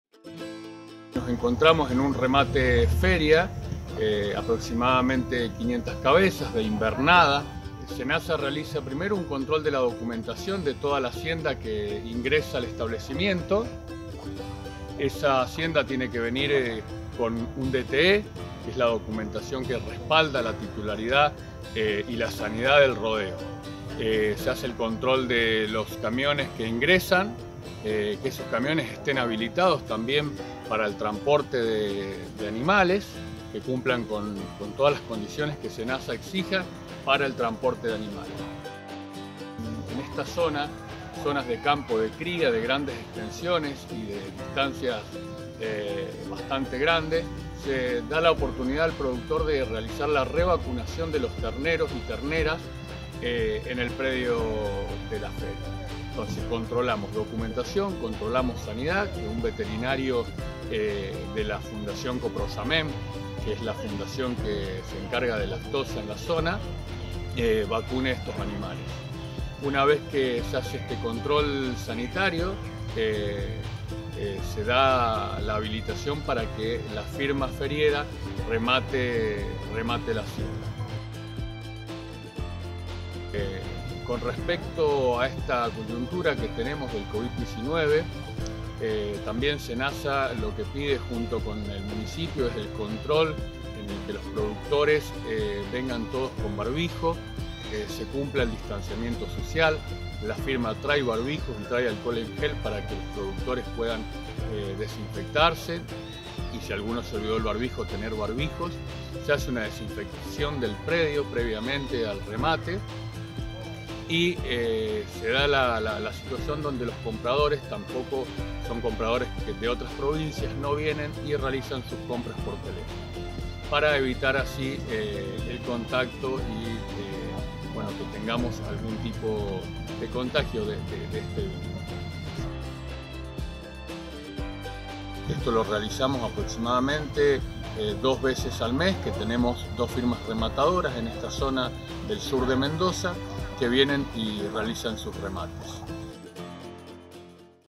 Remates de hacienda seguros
El Senasa controla que  se cumpla con los protocolos de prevención contra la COVID 19 y el estado sanitario de los animales que entran a remate